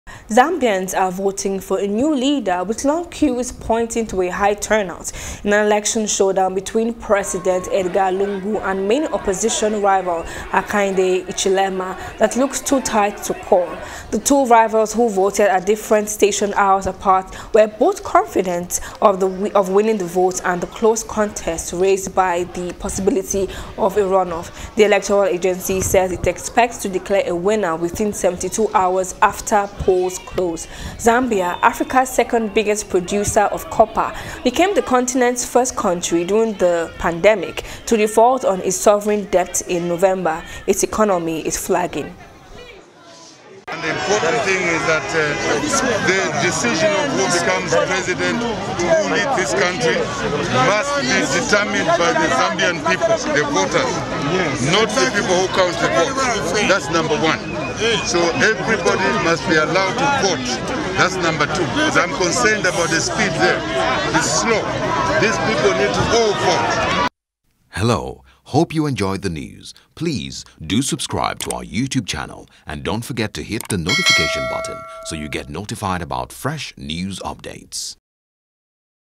Zambia: Crowds Cheer As Opposition Leader Hichilema Casts Vote | AFRICAN
Zambians are voting for a new leader, with long queues pointing to a high turnout in an election showdown between President Edgar Lungu and main opposition rival Hakainde Hichilema that looks too tight to call.